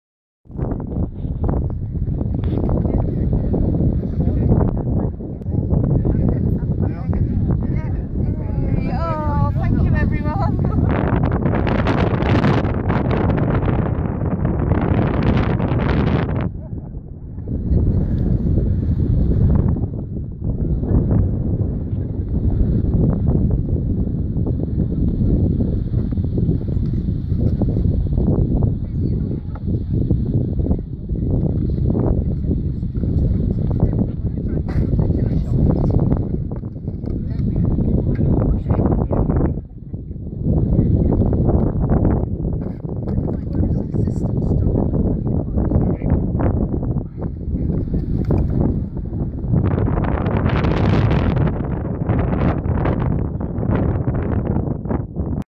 Hadrian's Wall on the Roman Military Way by Off Road Mobility Scooter
Hadrian's Wall above Housesteads Roman Fort on the Roman Military